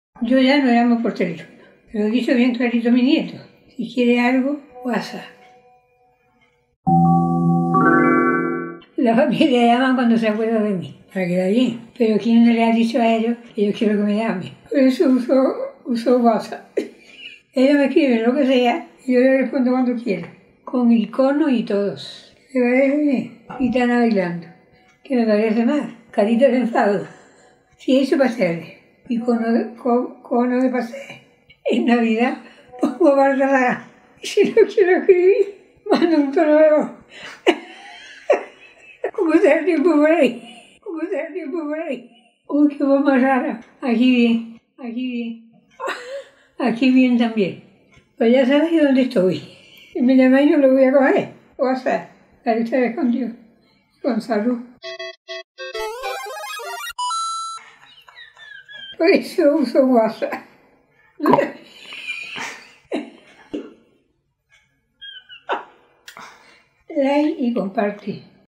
CATALINA 2.0 "guasap"
CATALINA IS BACK!
Y esta vez viene a hablarnos del WhatsApp

PD2: todos sus vídeos en YouTube